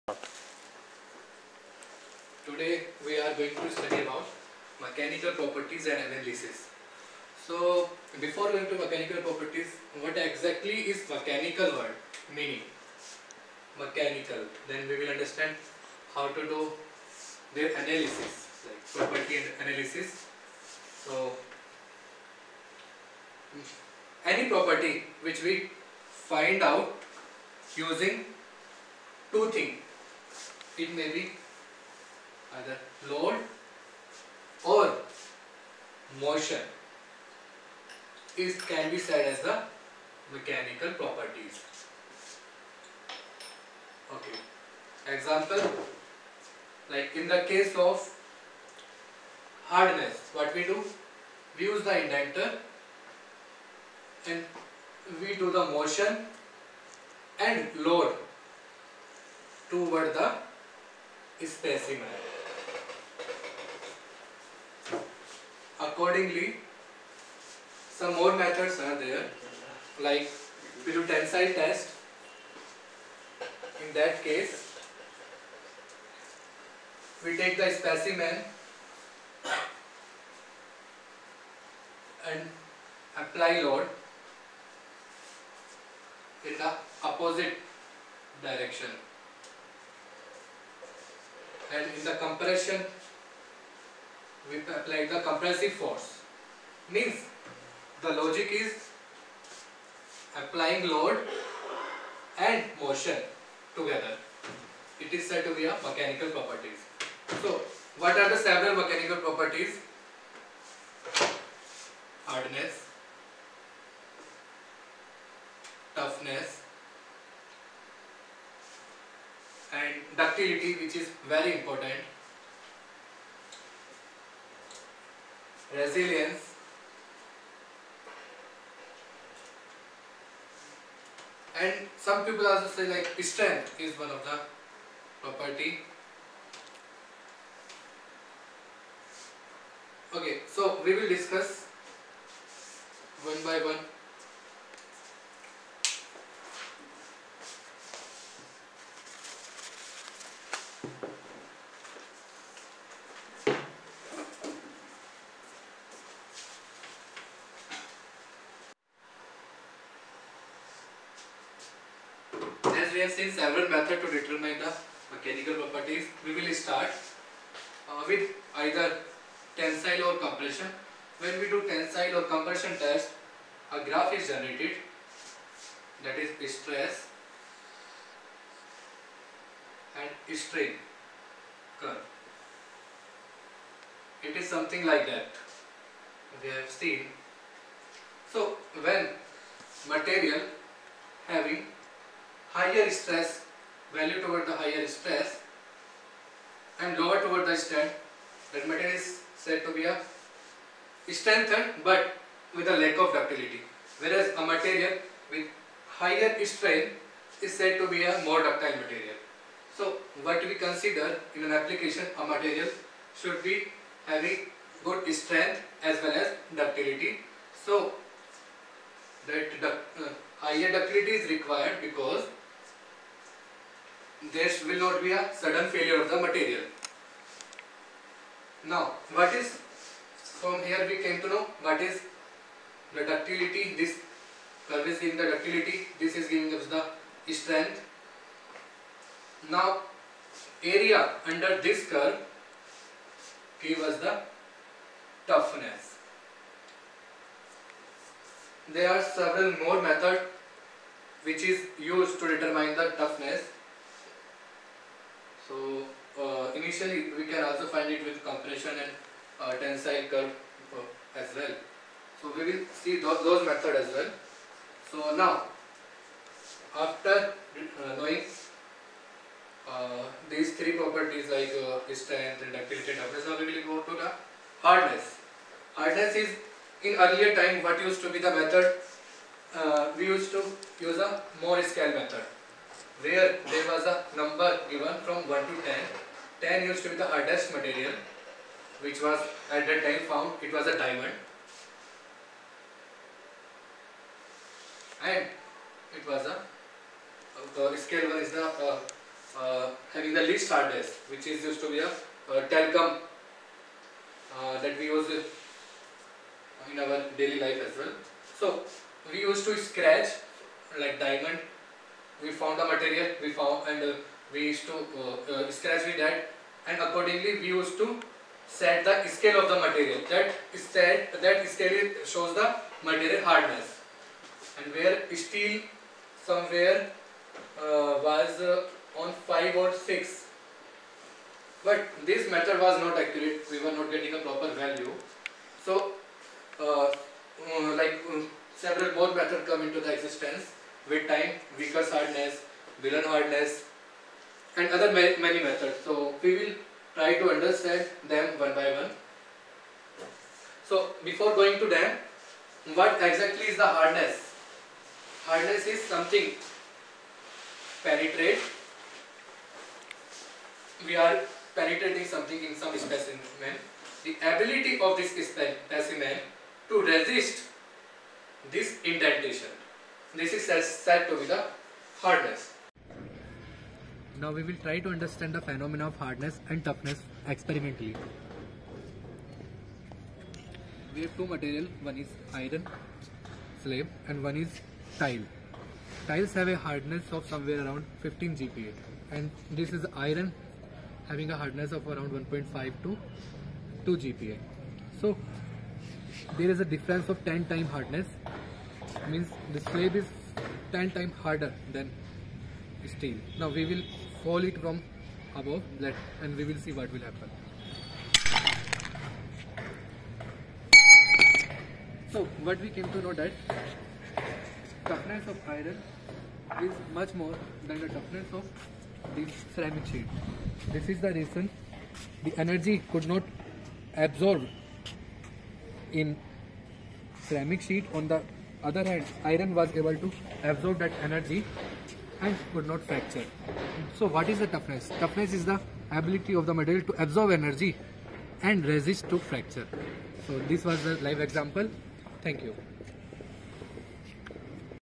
Introduction to Mechanical Properties and Analysis Part 1
Mechanical properties are also used to help classify and identify the material. The most common properties are strength, ductility, hardness, impact resistance, and fracture toughness.
Theoretically experimentally tried to explain the fundamental of hardness and toughness.